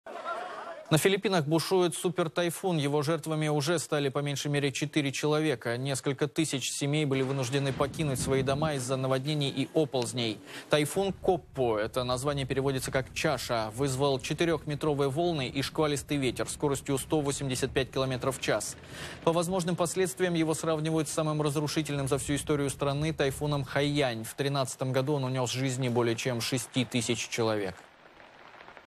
На Филиппинах бушует супертайфун
Его жертвами уже стали, по меньшей мере, четыре человека. Несколько тысяч семей были вынуждены покинуть свои дома из-за наводнений и оползней.